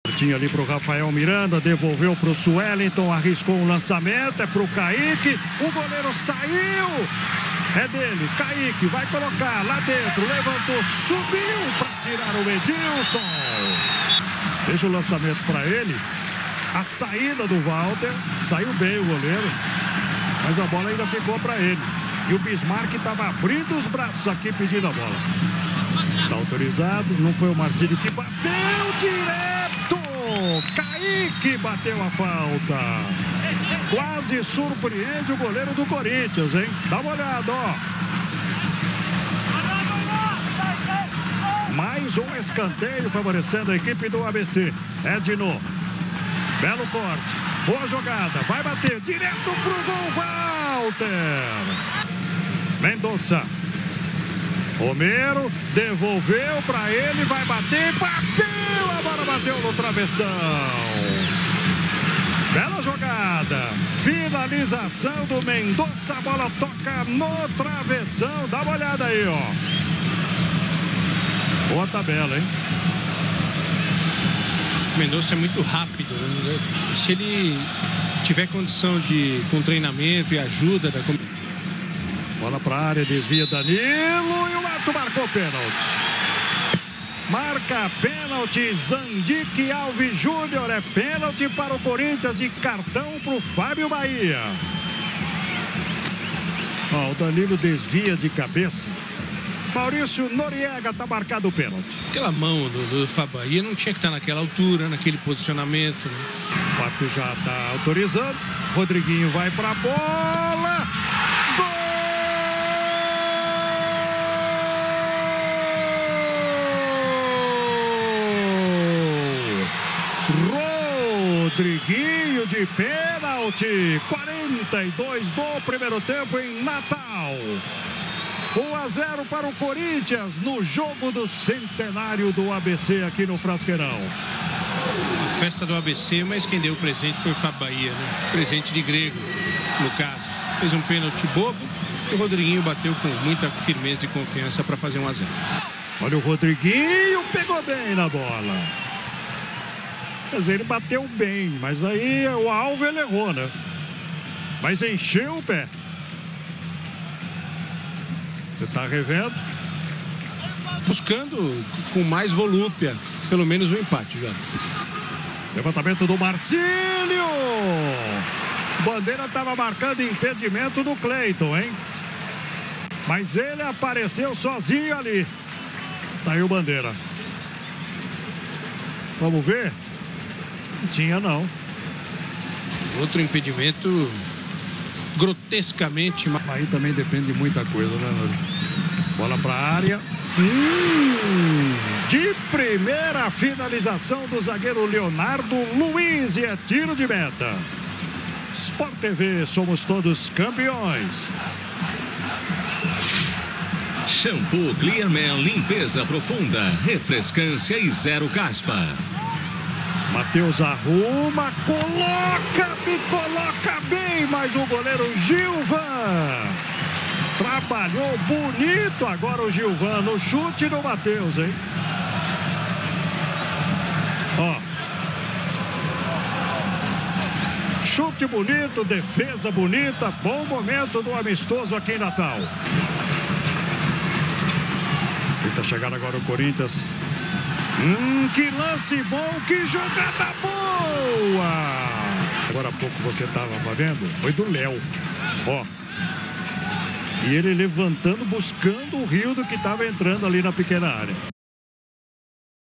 abc 0x1 CORINTHIANS
amistoso interestadual
22/07/2015- quarta- 19h30
Frasqueirão Natal RN
arb:Zandick Gondim Alves Júnior, Leandro Lincoln Santos Neves, Luis Carlos de França Costa

abc: 1-Saulo (12-Gilvan int), 2-Reginaldo (13-Igor Julião 18'2ºt), 4-Leandro Amaro   (14-Luizão int), 3-Sueliton (15-Leonardo Luiz int) e  6-Marcílio (16-Jussandro 18'2ºt) ,  5-Fábio Bahia( 22-Erivélton int) (23-Rafael Oliveira 24'2ºt), 8-Rafael Miranda  (17-Michel int),  7-Neto  (20-Rafinha int), 10-Edno   (21-Cleyton  int), 11-Bismark (19-Wellington Bruno int), 9-Kayke  (18-Jardel int)  Téc:Toninho Cecílio

CORINTHIANS: 27-Walter (32-Matheus Vidotto 20'2ºT), 2-Edílson (13-Samuel 46'2ºT), 42-Edu Dracena, 3-Yago e 22-Marciel (29-Rodrigo Sam 43'2ºt), 5-Ralf , 19-Matheus Pereira, 26-Rodriguinho (  15-Matheus Vargas 40'2ºT), 20-Danilo (33-Rildo 19'2ºt), 30-Mendoza (31-Léo Jabá 28'2ºT) e 11-Romero (14-Gustavo Viera 33'2ºT ). Téc Tite